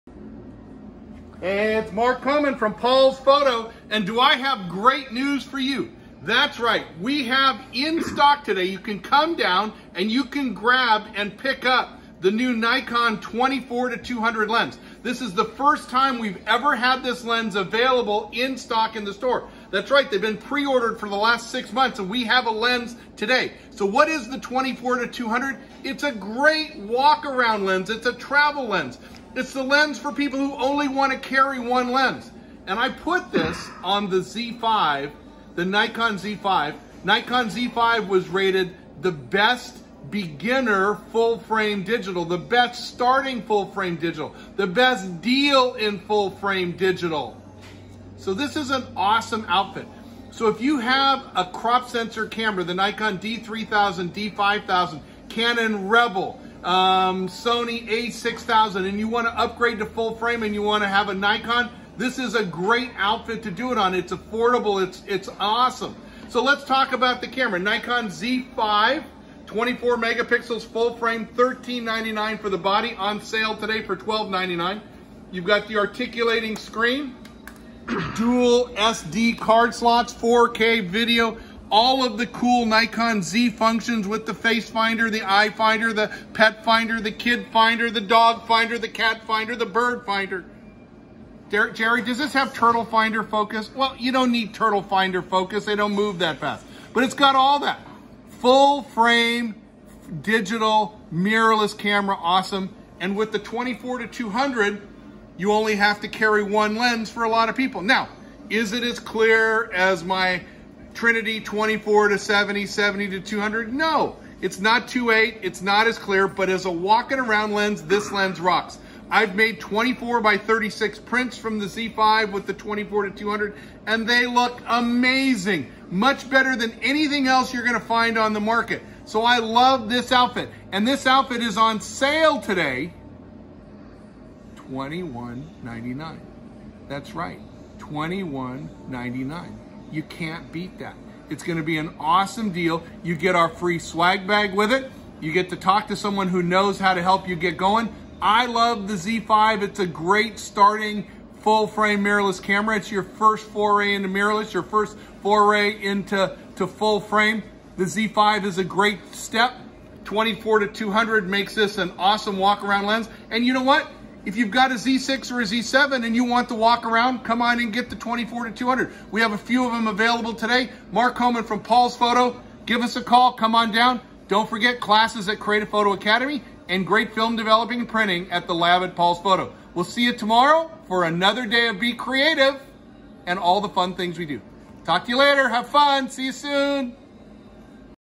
Rockin Startin Kit
Nikon Z 24-200 and Z5 makes a GREAT starter Mirrorless full frame kit.  C’mon down and let’s get you hooked up Pauls Photo